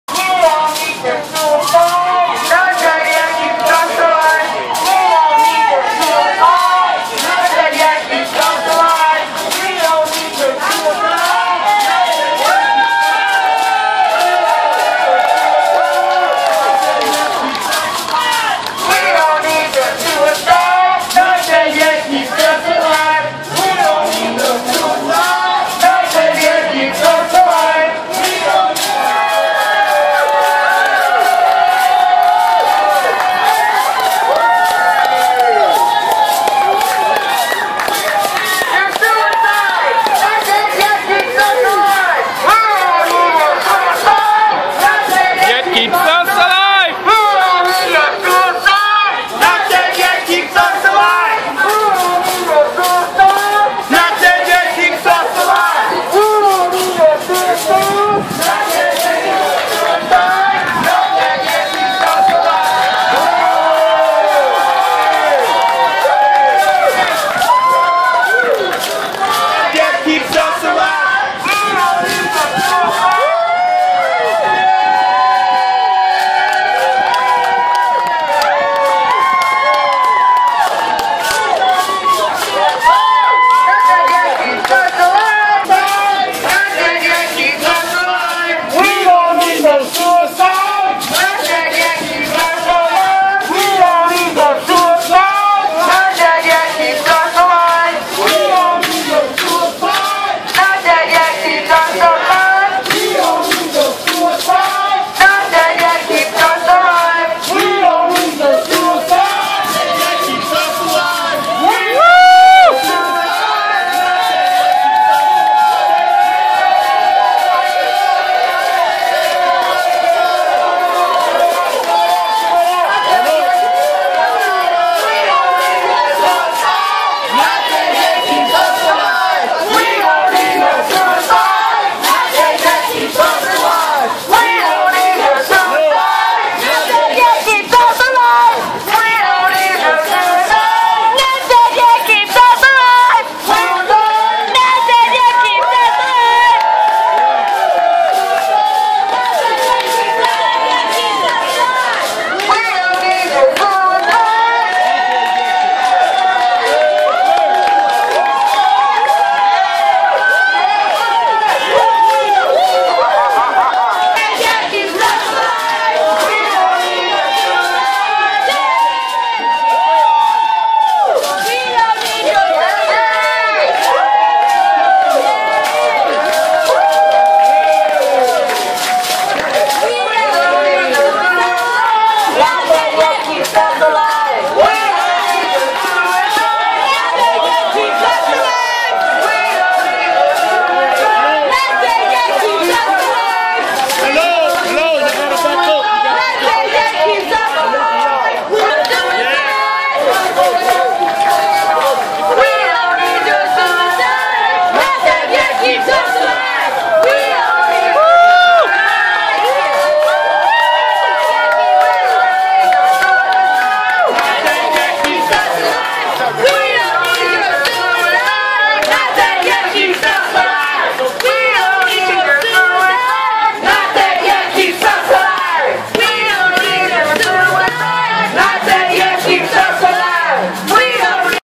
Police escorts Not Dead Yet members out of Embassy Suite hotel in Chicago
September 20, 2014. Not Dead Yet and ADAPT commandeered all public elevators inside the Embassy Suite hotel in protest of the World Federation of Right to Die Societies Conference. This was the third day of protest.

This video is of the brave souls who risked arrest and infiltrated the hotel, as they were escorted out of the hotel by police, one by one.

The audio band only contains cheering and chanting ("We don't need your suicide, Not Dead Yet keeps us alive")

A narrative of the 3-day event by Diane Coleman is available on the Not Dead Yet